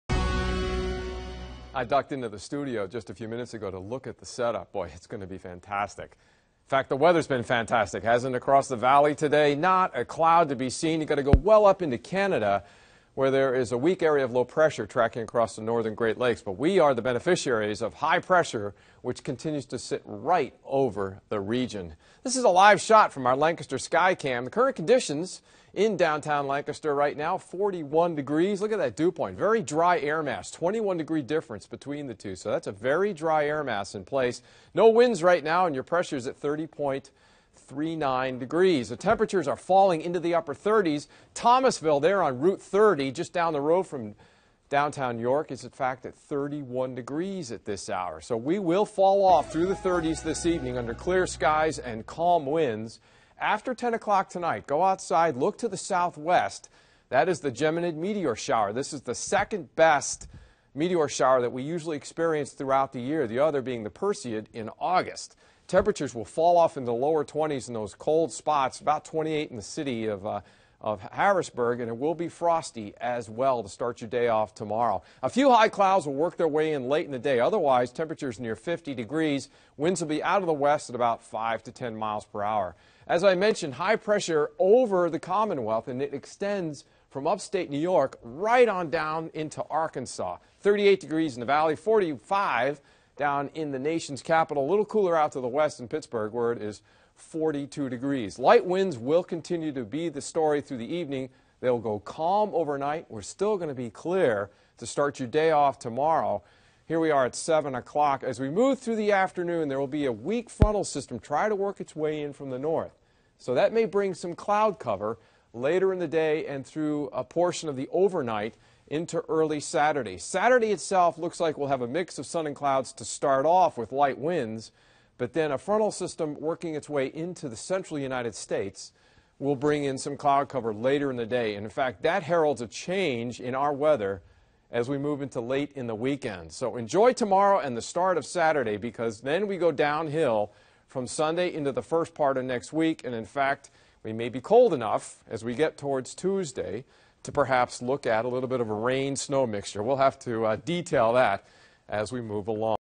Frosty night expected
A clear, chilly night is expected, which makes for great viewing for tonight's expected meteor shower.